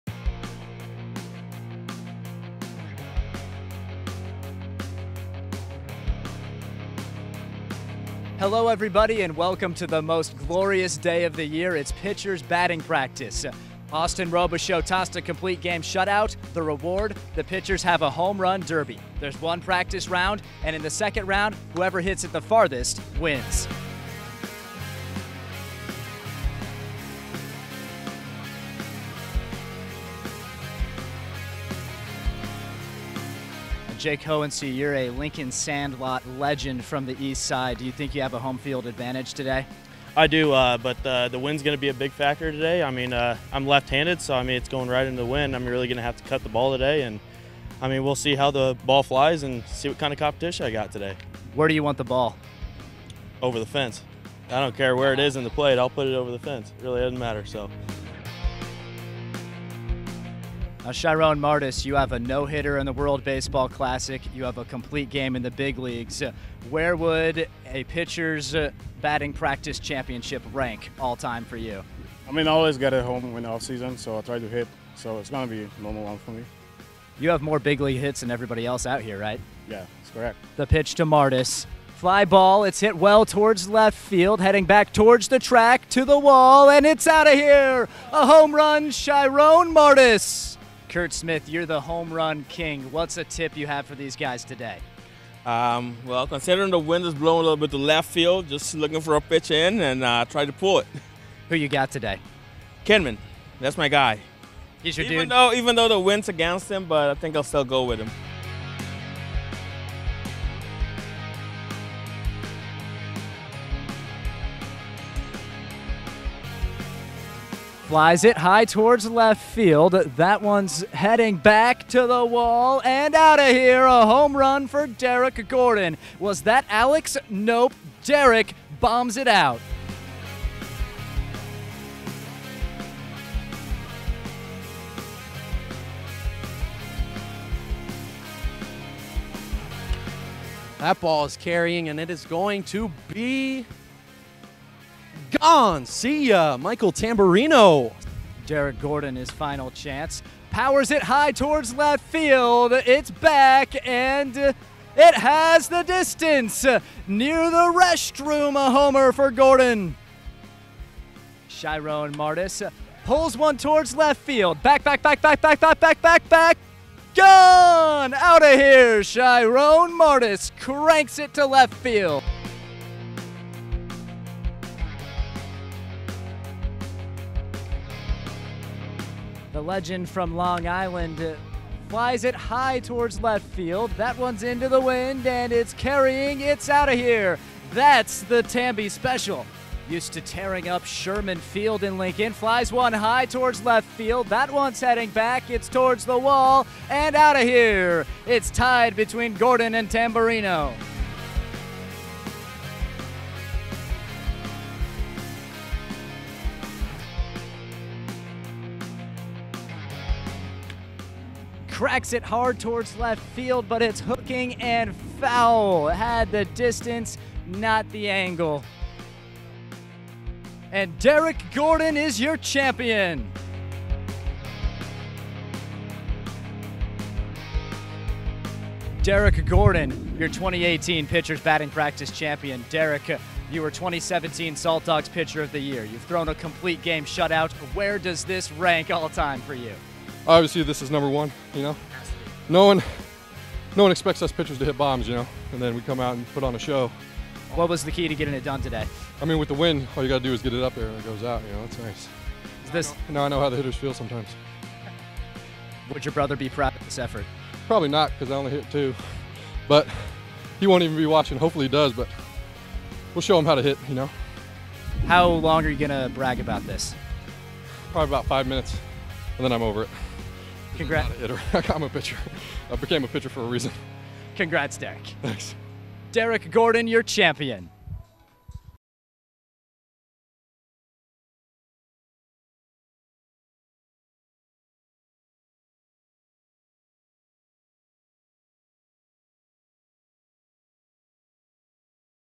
Pitcher's Batting Practice
Every pitcher thinks they can hit, but can they when they get the chance to prove it? The Saltdogs pitchers showed a little pop in the bat, but see who had the best long balls during the 2018 Pitcher's Batting Practice!